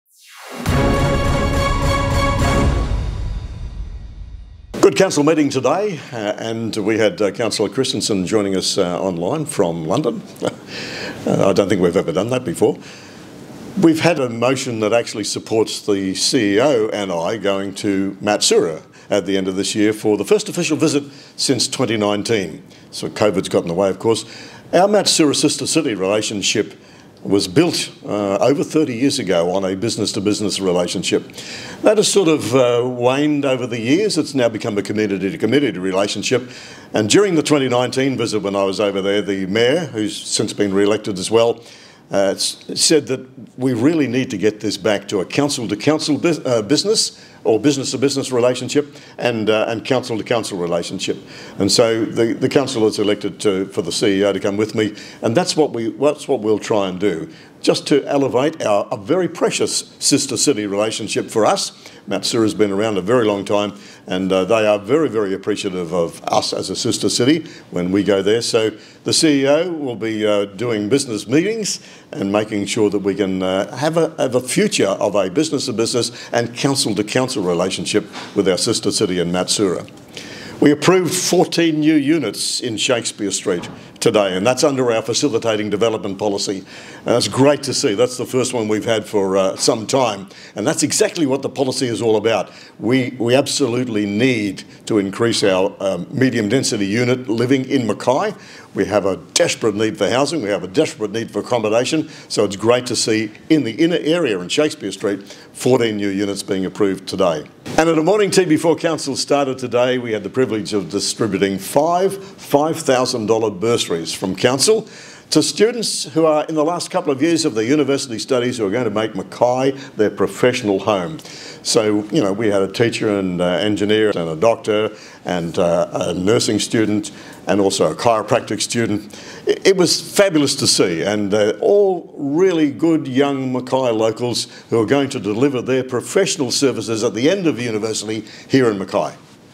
From the Chambers - July 24, 2024
Mayor Greg Williamson provides a wrap-up of the highlights from today’s ordinary meeting of council.